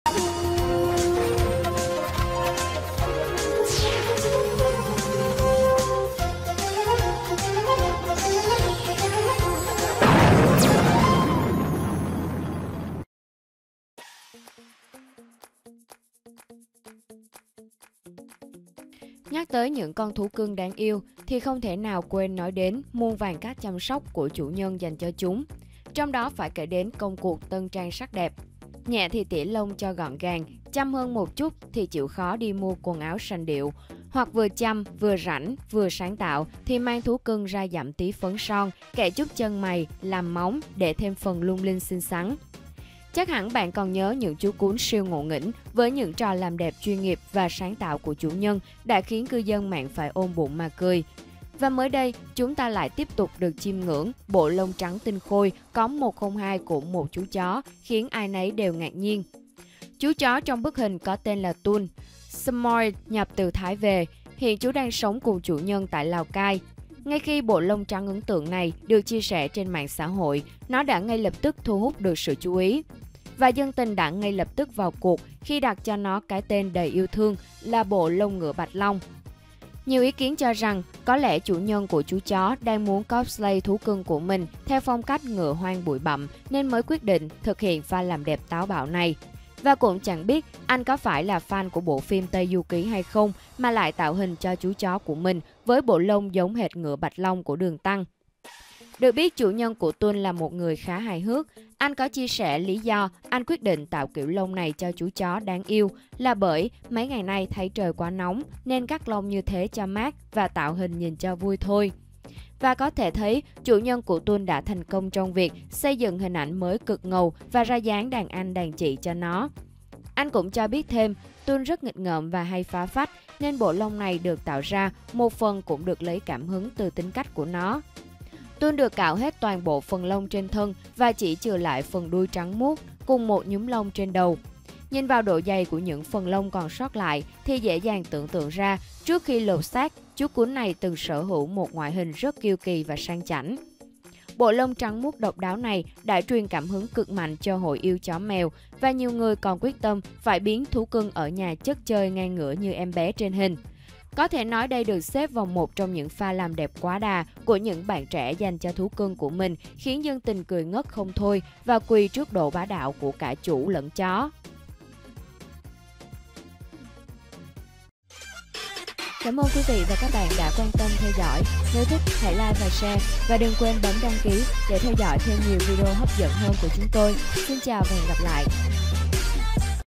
"Đứng hình" trước bộ lông y hệt ngựa Bạch Long của chú chó samoyed
Hình ảnh về bộ lông được cư dân mạng đặt cho cái tên đầy yêu thương là "ngựa Bạch Long" của một chú chó chắc chắn sẽ khiến bạn phải thốt lên đầy bất ngờ về độ "chịu chơi" của cặp đôi chủ tớ này đó nhé.